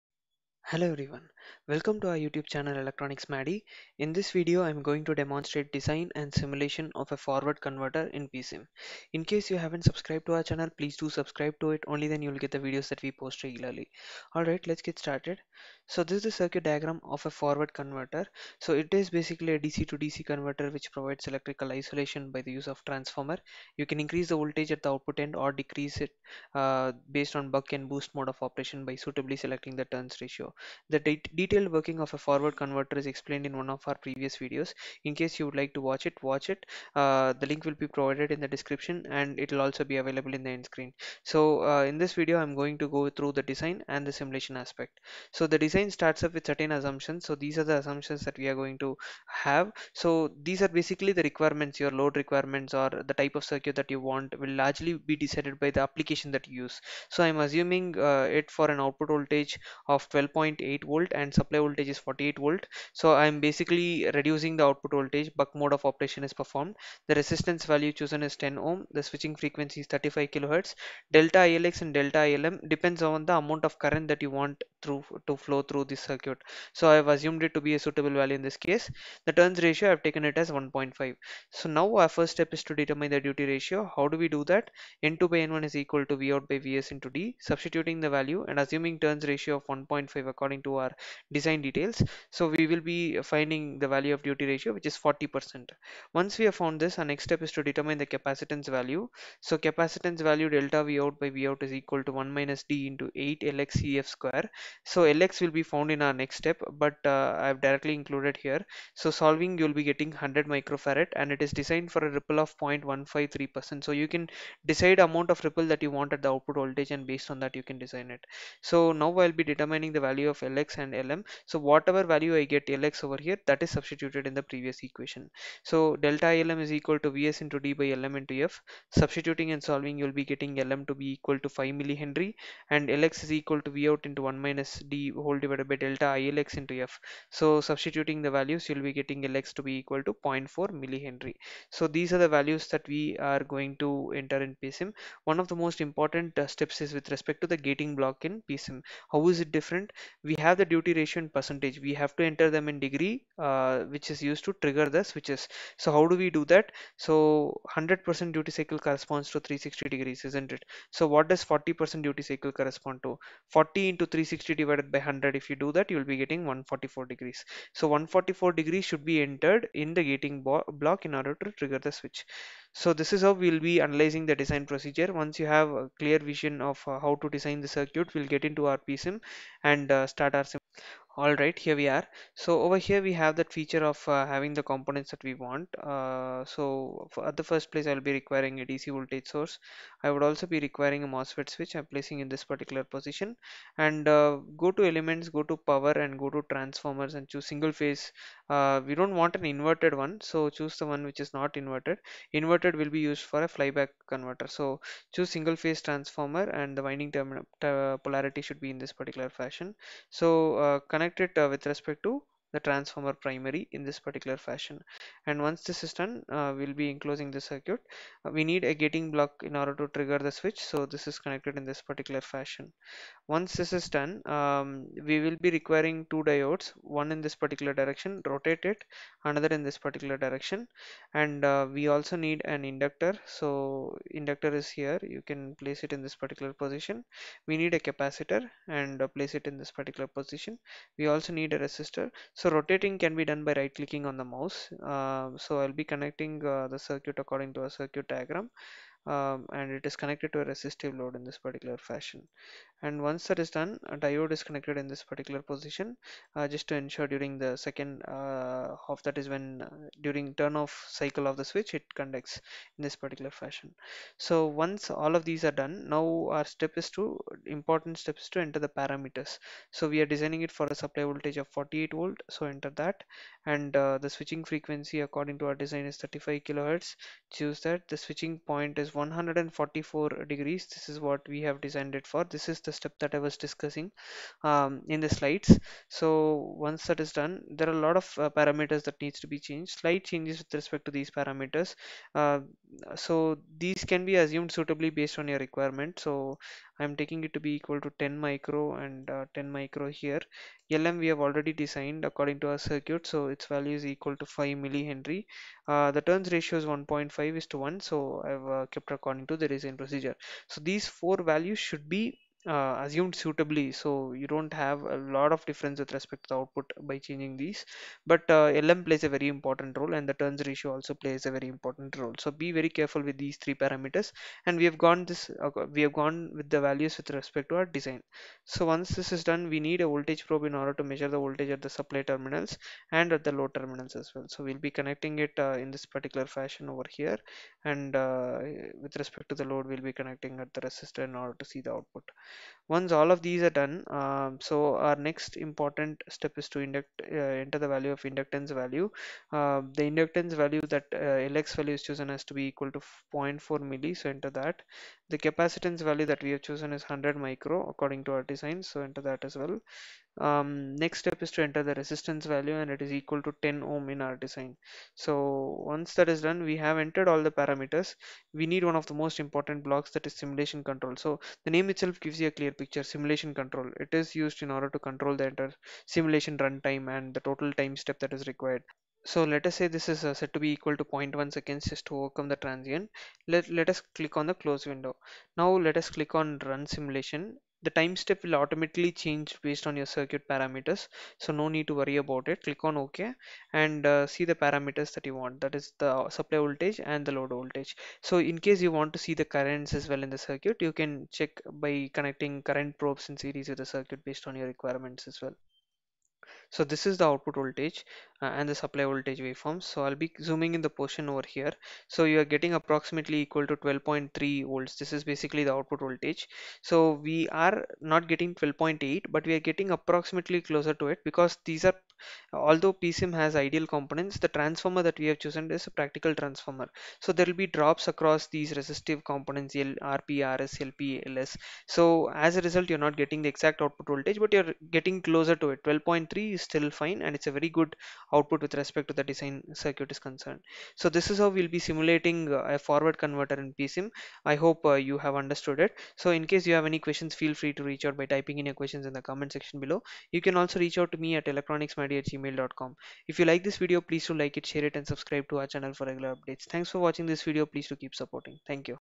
Design and Simulation of Forward Converter in PSIM | POWERSIM
This video demonstrates the design and simulation of the Forward Converter / isolated DC-DC Converter using PSIM (POWERSIM). One of the major advantages of the Forward converter is it provides electrical isolation used for protection purposes. The forward converter is used for high power rating in comparison with a flyback converter. The design involves a lot of assumptions and care has to be taken in assuming suitable value.